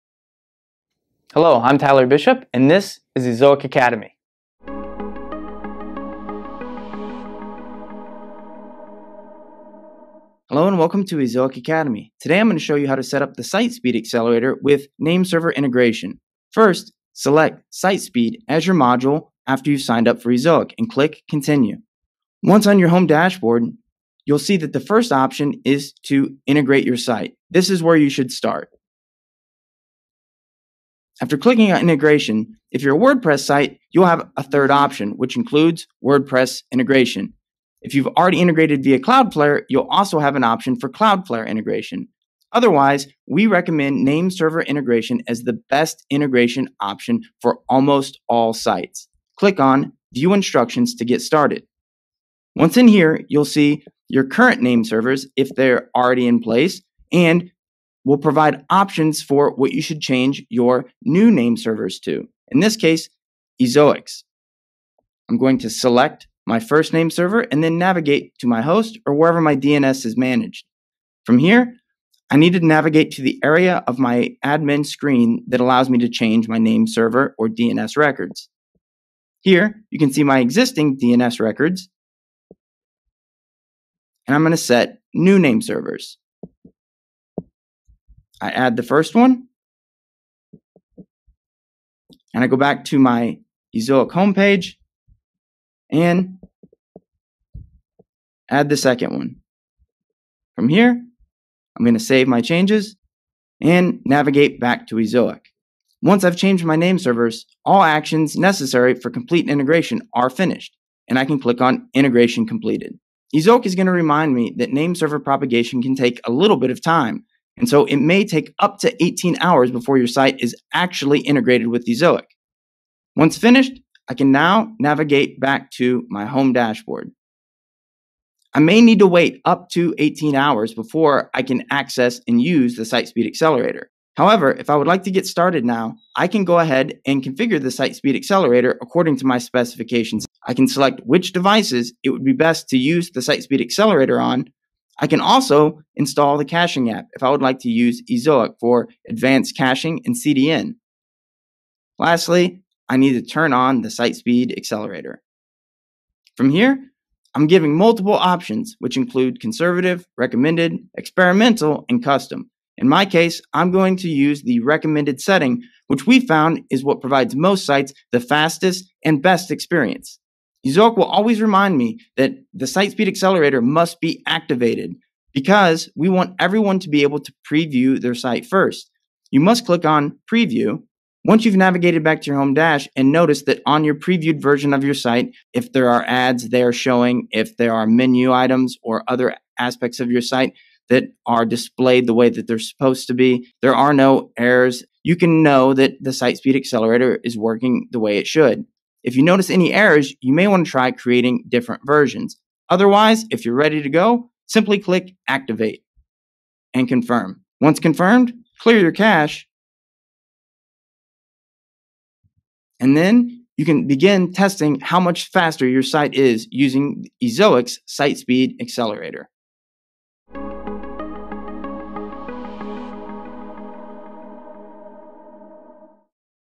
Get Started with Site Speed Accelerator via Nameserver Today (BEST OPTION)
Join Tyler as he goes over how to integrate with Ezoic through a quick nameserver change and then begin using the Site Speed Accelerator.

Ezoic’s Site Speed Accelerator instantly increases website speed, page load times, and PageSpeed scores. With one-click, a site’s mobile Google PageSpeed score will be 80+, guaranteed. No broken pages, no coding, and no troubleshooting.